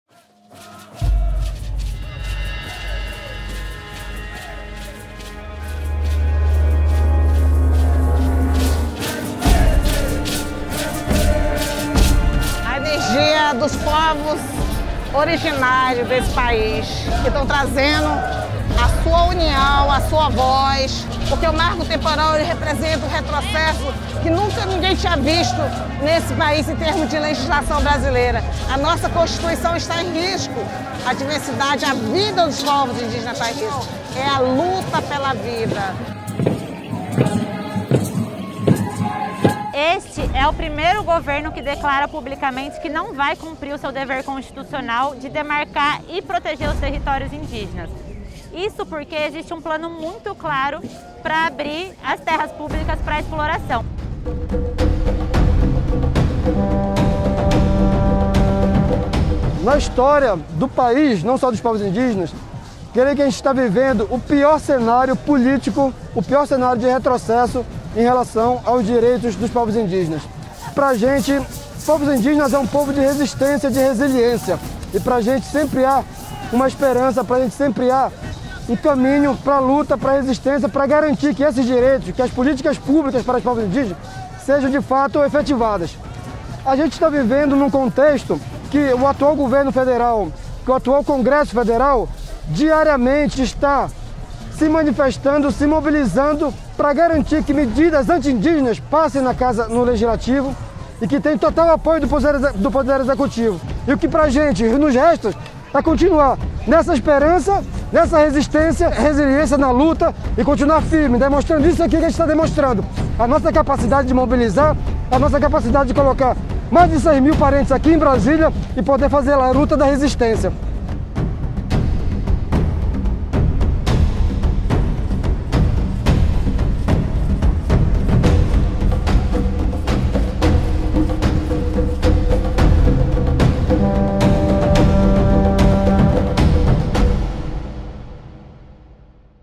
Primavera indígena
Vamos mobilizar o Brasil contra o Marco Temporal! Assine e convide outras pessoas para participarem do abaixo-assinado

“O marco temporal representa um retrocesso que ninguém nunca tinha visto neste país em termos de legislação brasileira. A nossa Constituição está em risco”.
Não podemos permitir que uma tese ruralista ameace de maneira seríssima os povos indígenas e o patrimônio natural brasileiro!